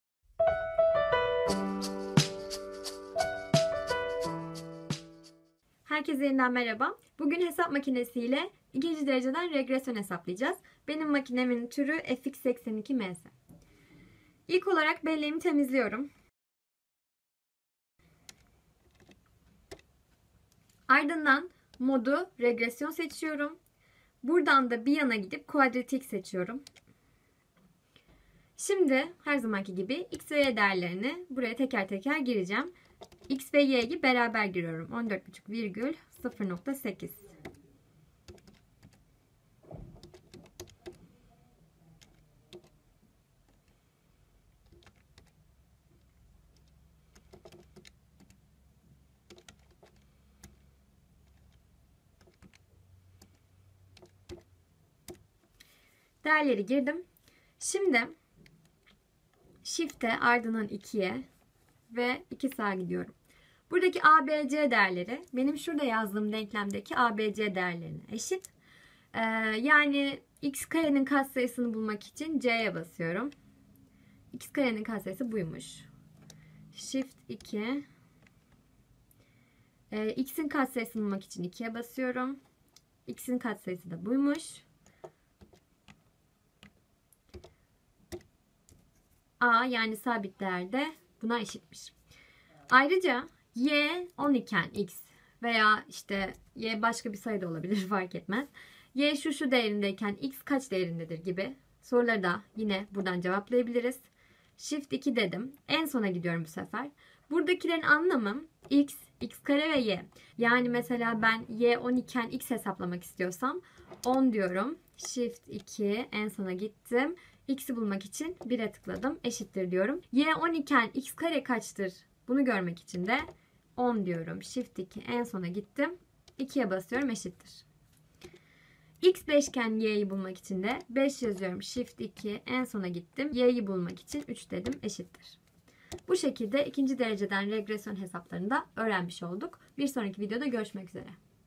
Bilimsel Hesap Makinesi Kullanımı 7 - 2.Dereceden Regresyon (Calculator Tutorial 7)
Hesap makinesiyle 2. dereceden regresyon problemi nasıl çözülür?
Casio fx-82MS Bilimsel Hesap Makinesi
Quadratic Regression by calculator